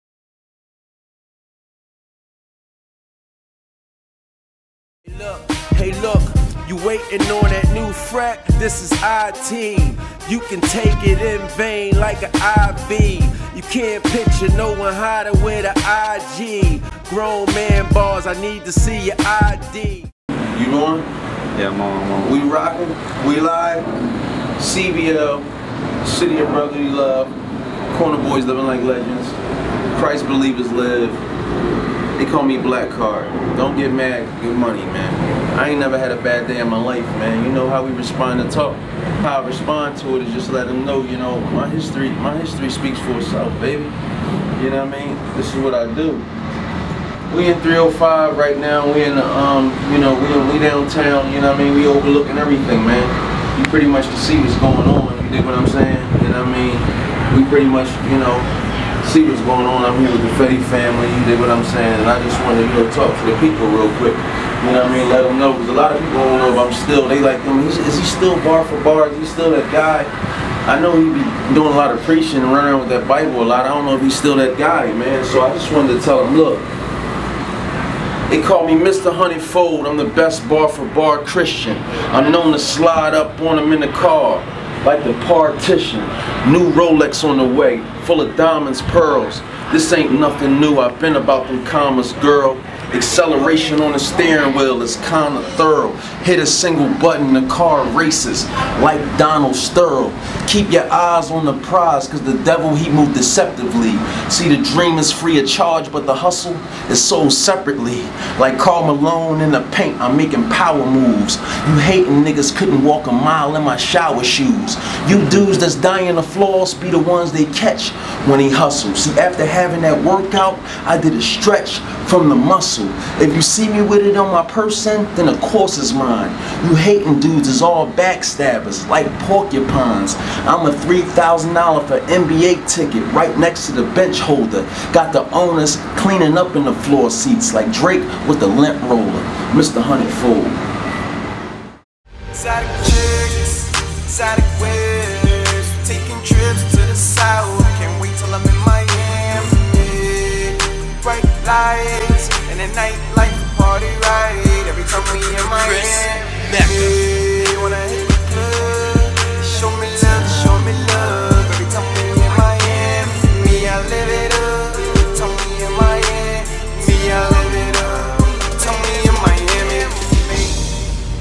Freck Billionaire Balcony Freestyle 2014
West Philly Freck showcasing some bars on PentHouse Balcony